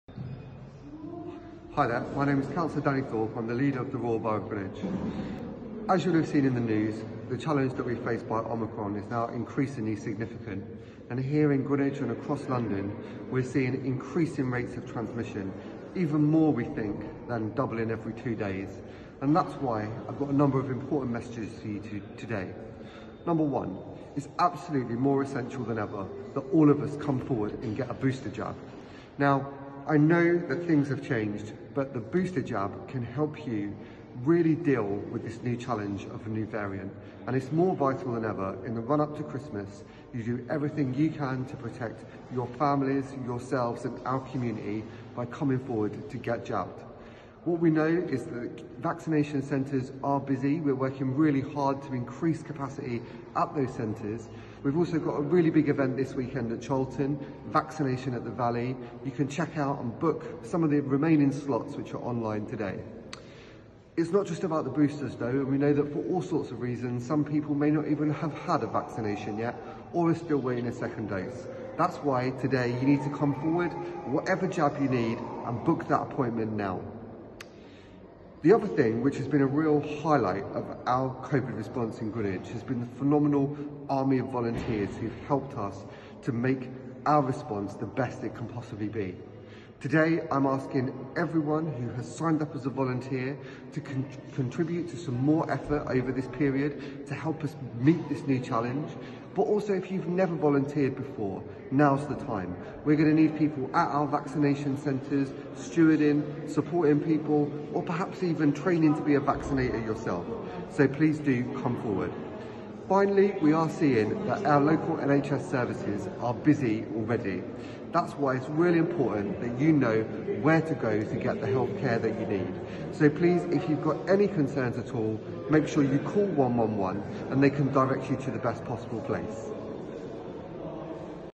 Just some of the reasons why we should all get boosted and vaccinated
Cllr Danny Thorpe, the Leader of the Royal Borough of Greenwich, talks about the Omicron variant, the importance of getting your booster and vaccination at The Valley, and how we can all play our part.